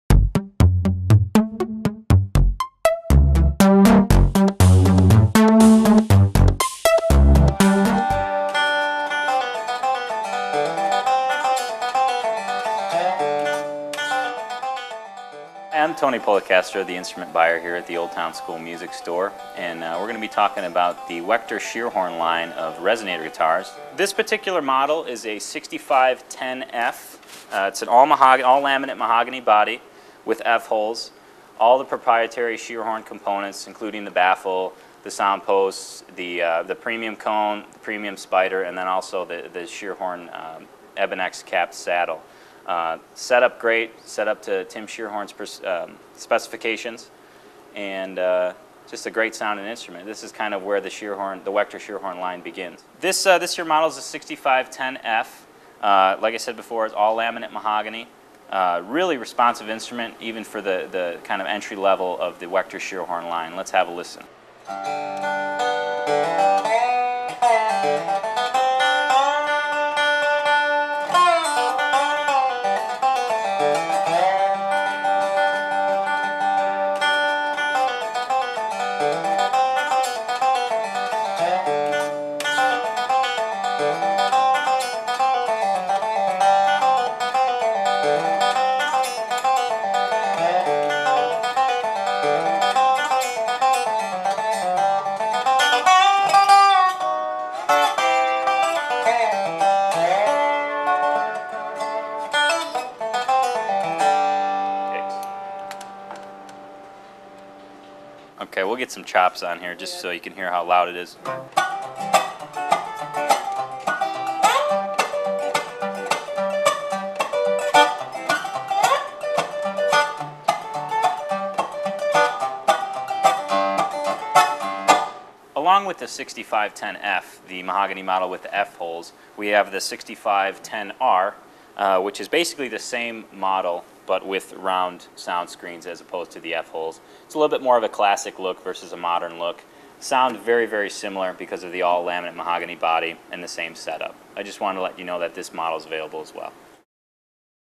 Wechter Scheerhorn 6510 Series Resonators At Old Town School Music Store
Wechter Scheerhorn Resonators hold a special place in Tony Polecastro's heart, and as Old Town School Music Store's guitar buyer, he was more than happy to give us a tour of what these sweet-sounding dobros have to offer

The 6510 series feature a laminate mahogany top and sides for a more mellow sound. The "R" models have round, resonator screens above the resonator cone, and the "F" series just can't stop swearing!